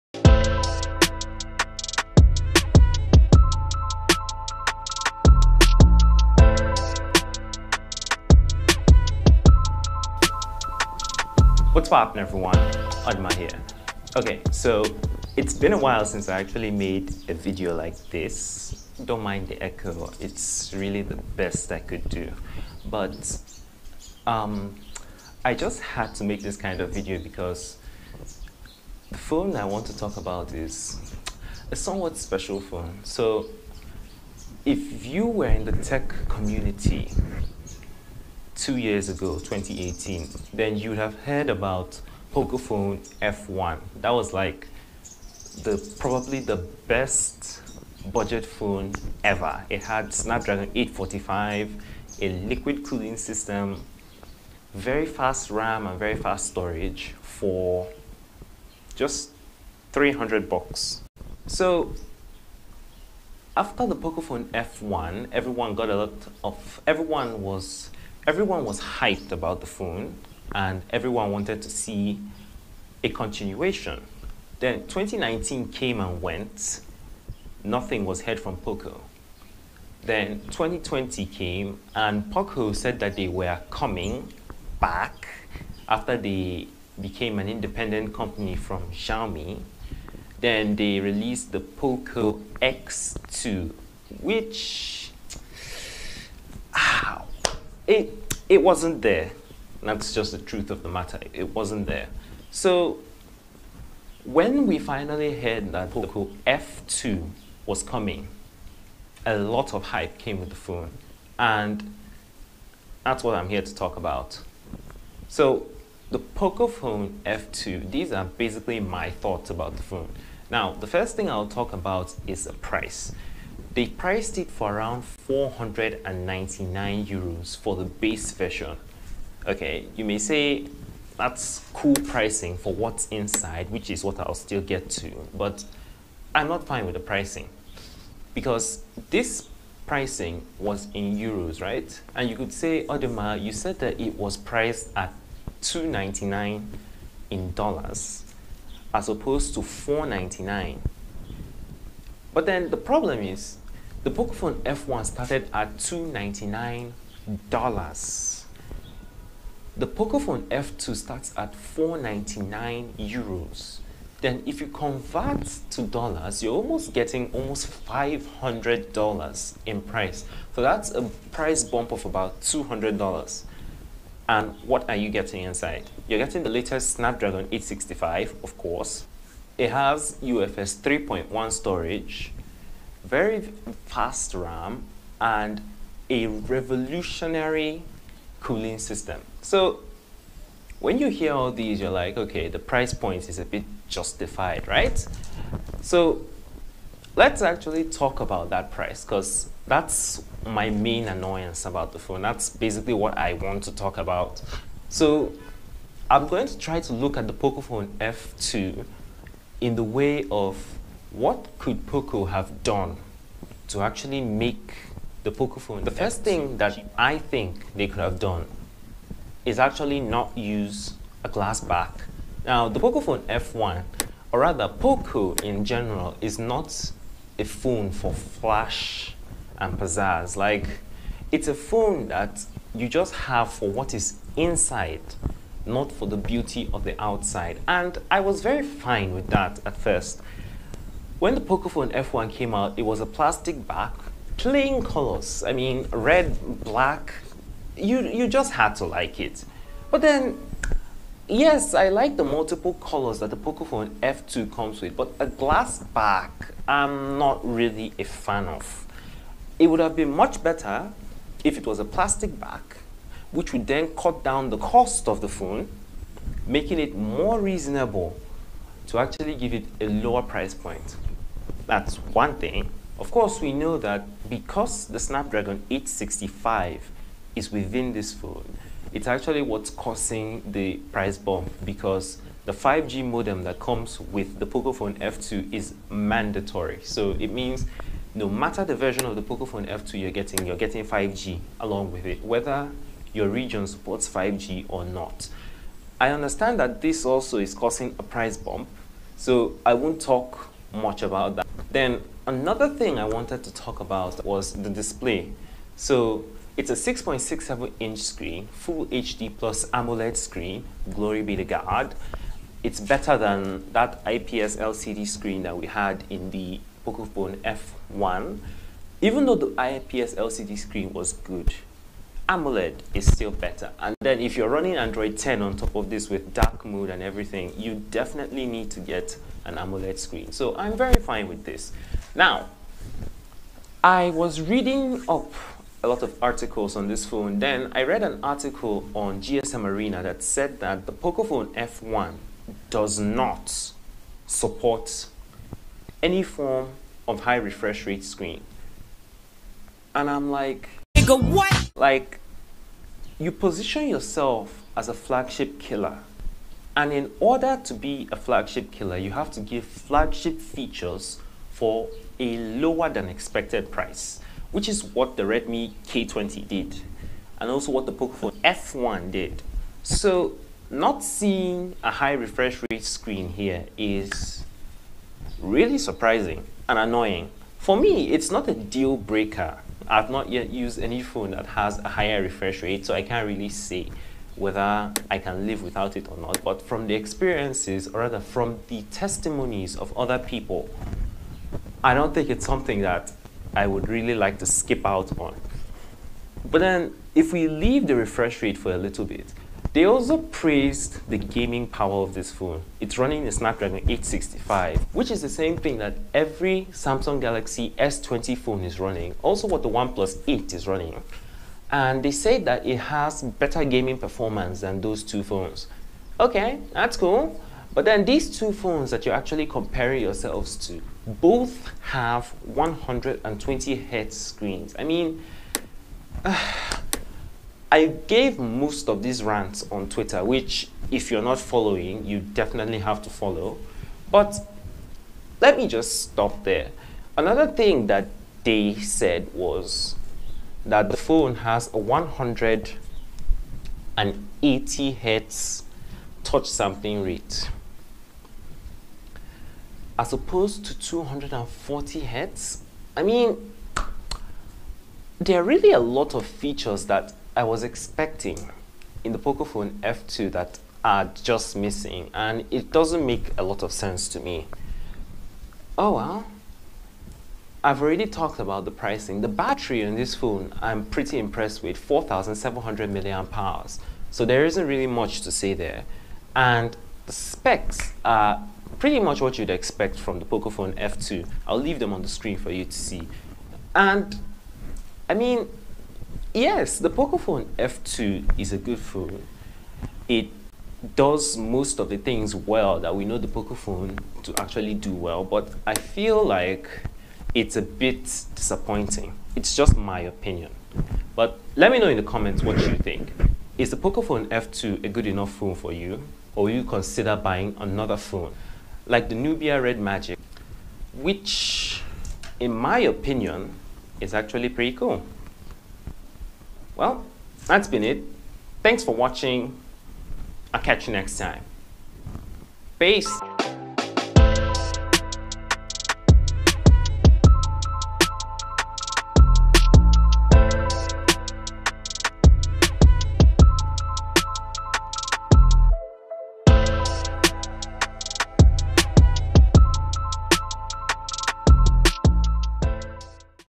Poco F2 Pro Impressions: Not Really A Flagship Killer!
Back in 2018, the Pocophone F1 launched and was known as one of the budget phones of all time with all the flagship bells and whistles for $300.

Now, 2 years later comes the Pocophone F2 and to be honest... I'm not very impressed by.

THESE are my honest thoughts, as a Nigerian who loves tech on the continuation of a phone many people love.

VIDEOS WILL BE COMING WEEKLY.